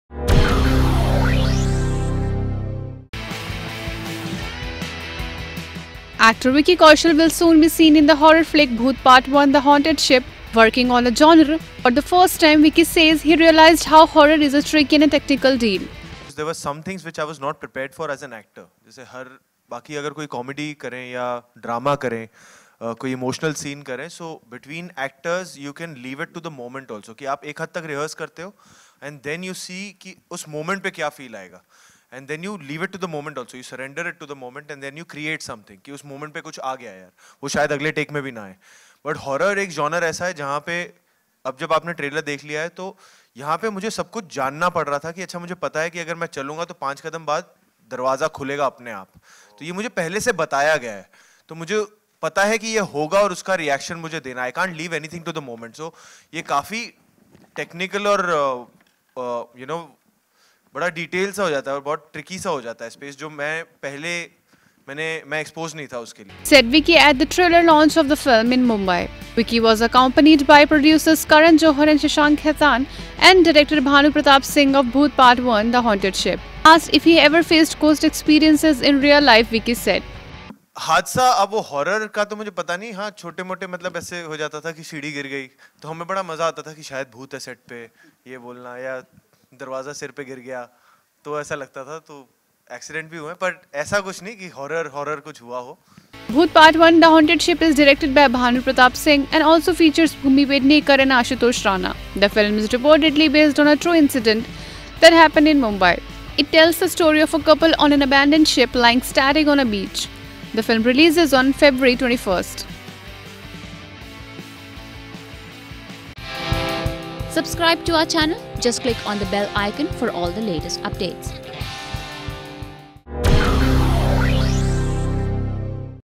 Vicky Kaushal: Horror is a tricky and technical genre
Actor Vicky Kaushal will soon be seen in the horror flick "Bhoot Part One: The Haunted Ship". Working in the genre for the first time, Vicky says he realised how horror is a tricky and technical deal.